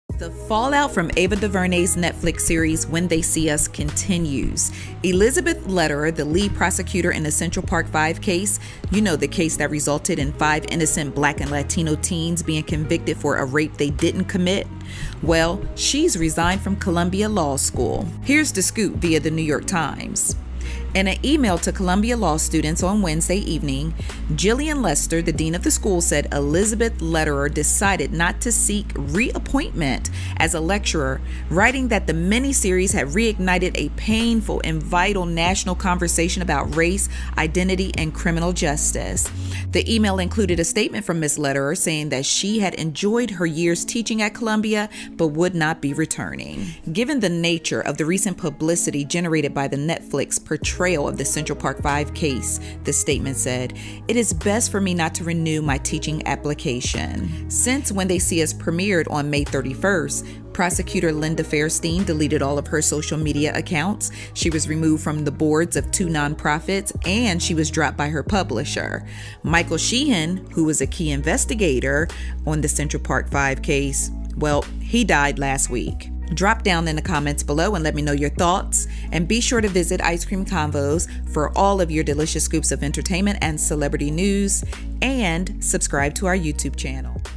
Central Park Five Lead Prosecutor Elizabeth Lederer Resigns From Columbia Law School
ElizabethLederer, the lead prosecutor in the CentralParkFive case that resulted in five innocent black and latino teens being convicted for a rape they did not commit, has resigned from ColumbiaLawSchool.

💗🍦 Shop 🍦💗

💗🍦 My Equipment 🍦💗